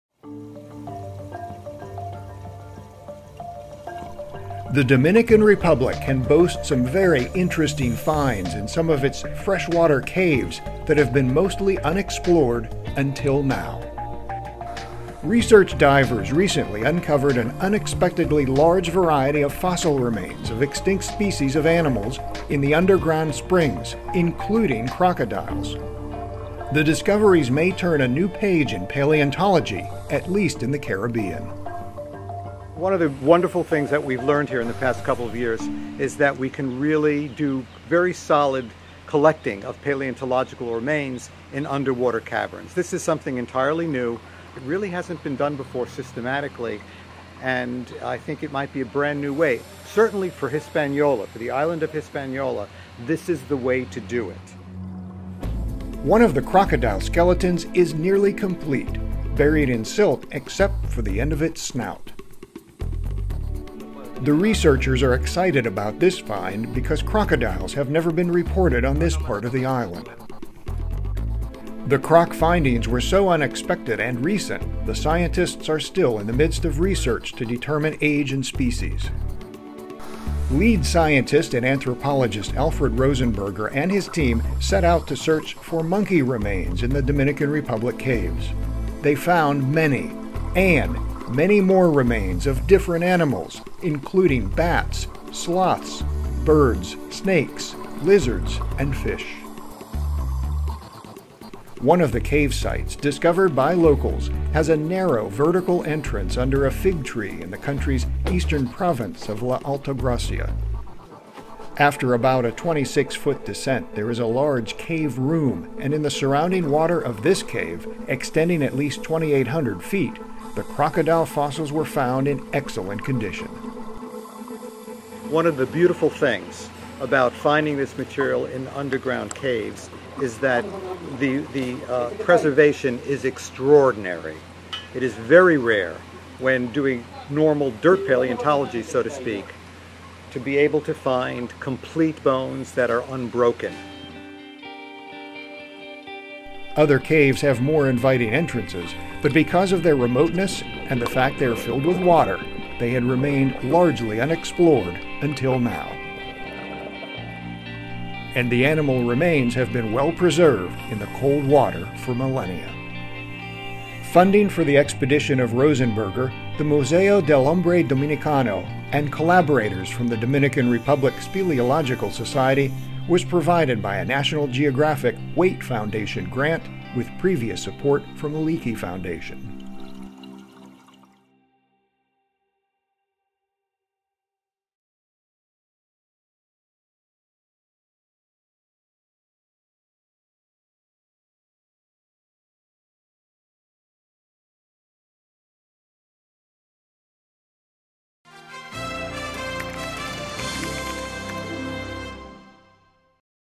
Croc Fossil Discovered in Cave | National Geographic
September 26, 2011 — A skeleton of a possibly extinct crocodile is among several fossil surprises unearthed in freshwater caves of the Dominican Republic, paleontologists say.

Original video produced by Phillip Lehman, Dominican Republic Speleological Society and Aquavista Films